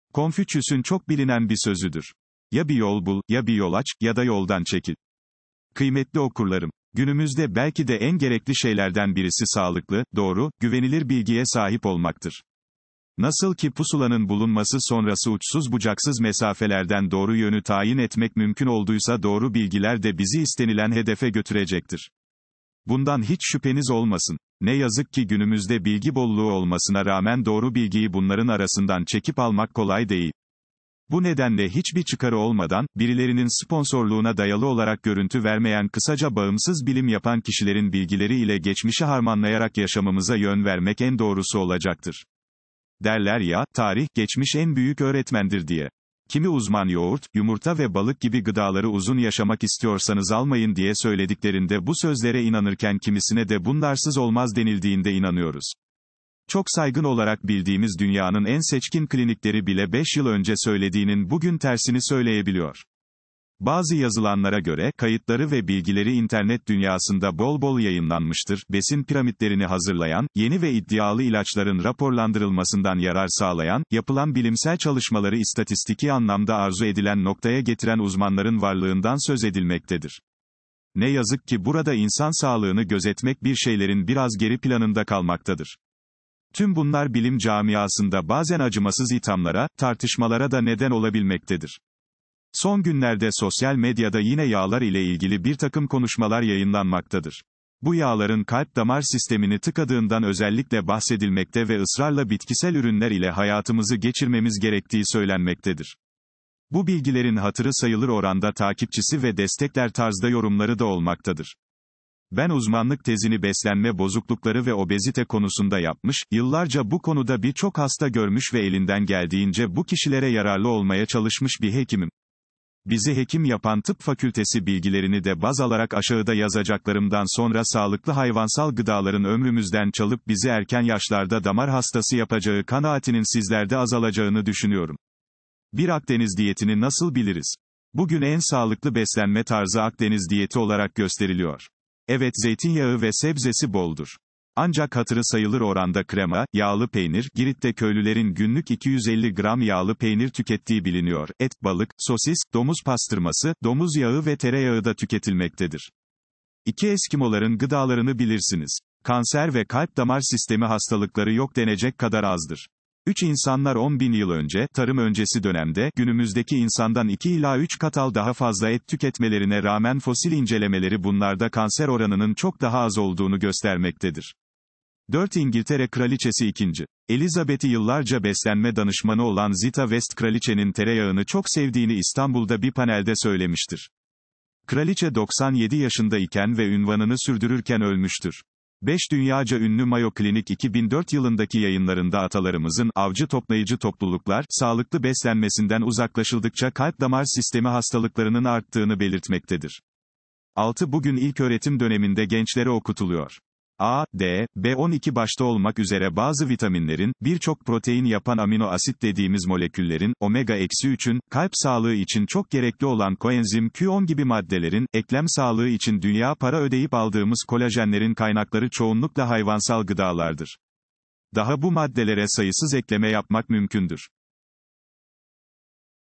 Ya bir yol bul ya da yoldan çekil
Dr. Mete Ekşioğlu - Ya bir yol bul ya da yoldan çekil - 15 Ocak 2024 Pazartesi - Konfüçyüs’ün çok bilinen bir sözüdür. Ya bir yol bul, ya bir yol aç, ya da yoldan çekil.
Kıymetli okurlarım. Günümüzde belki de en gerekli şeylerden birisi sağlıklı, doğru, güvenilir bilgiye sahip olmaktır. Nasıl ki pusulanın bulunması sonrası uçsuz bucaksız mesafelerden doğru yönü tayin etmek mümkün olduysa doğru bilgiler de bizi istenilen hedefe götürecektir. Bundan hiç şüpheniz olmasın. Ne yazık ki günümüzde bilgi bolluğu olmasına rağmen doğru bilgiyi bunların arasından çekip almak kolay değil. Bu nedenle hiç bir çıkarı olmadan, birilerinin sponsorluğuna dayalı olarak görüntü vermeyen kısaca bağımsız bilim yapan kişilerin bilgileri ile geçmişi harmanlayarak yaşamımıza yön vermek en doğrusu olacaktır. Derler ya, tarih, geçmiş en büyük öğretmendir diye…
Kimi uzman yoğurt, yumurta ve balık gibi gıdaları uzun yaşamak istiyorsanız almayın diye söylediklerinde bu sözlere inanırken kimisine de...